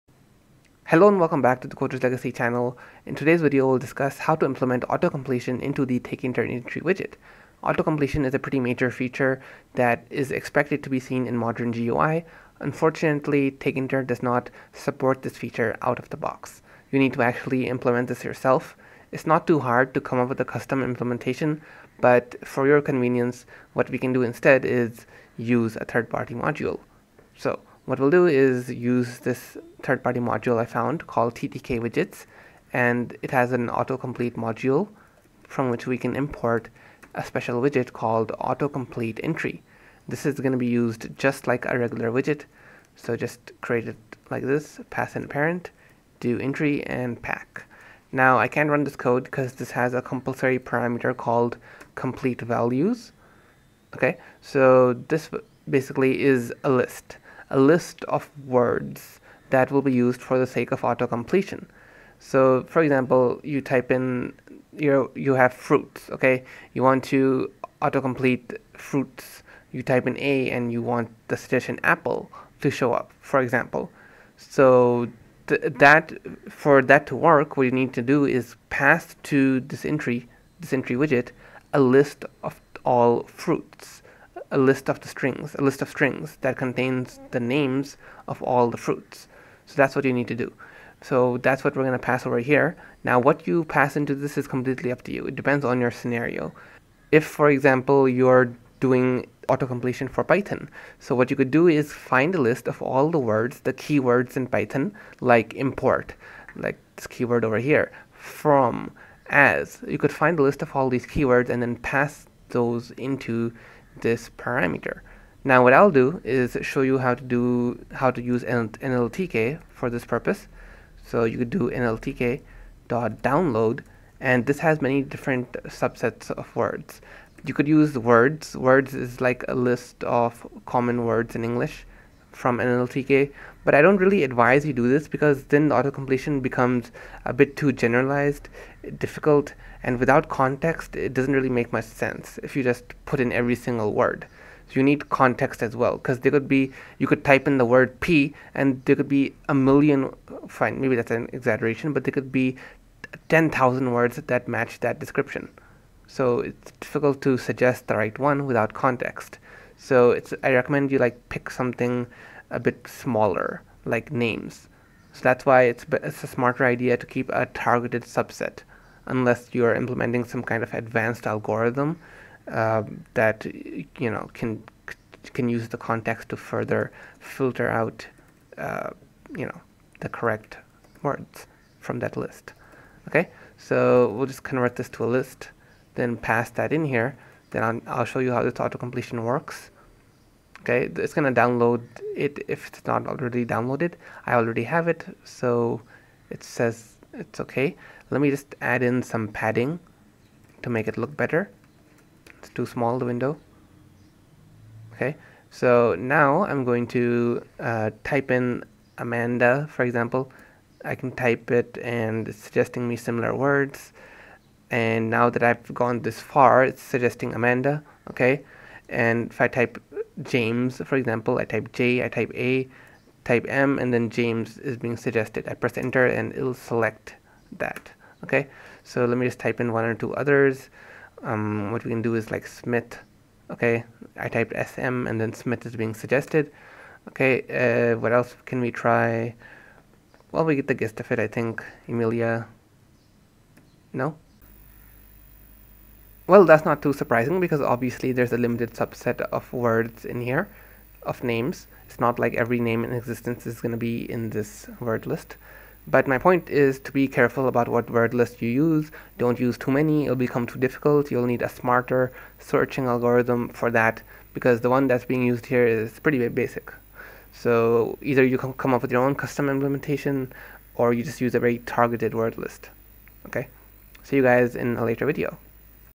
AutoComplete Entry Widget in Tkinter (Autofill suggestions)
Learn how to make an entry widget which offers automatic autofill suggestions while typing.